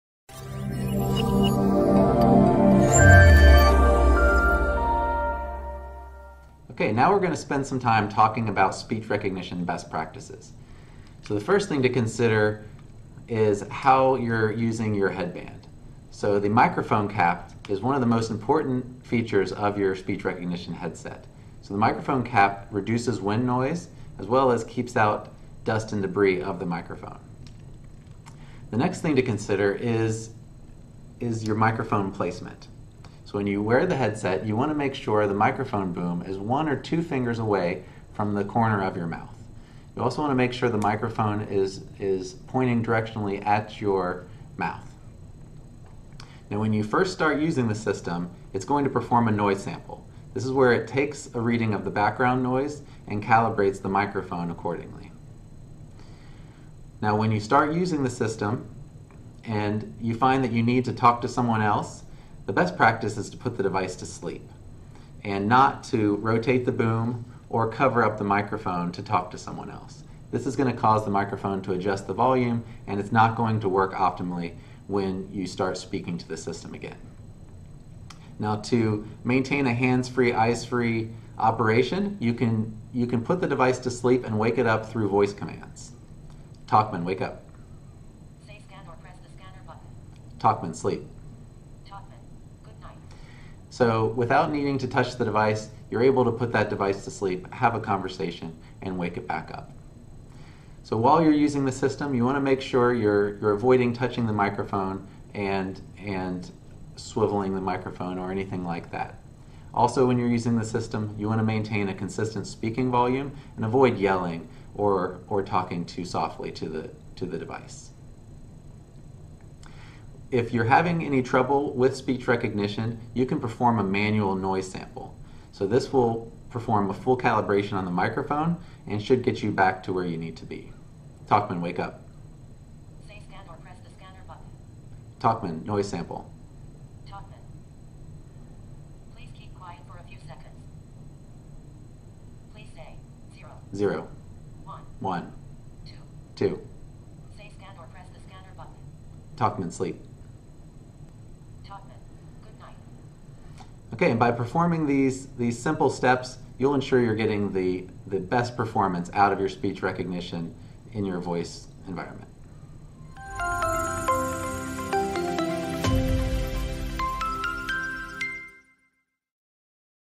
Honeywell Vocollect Talkman and SRX3 - Speech Recognition Best Practices
Provide best practices for obtaining optimal speech recognition using the Talkman A700x and SRX-3 headset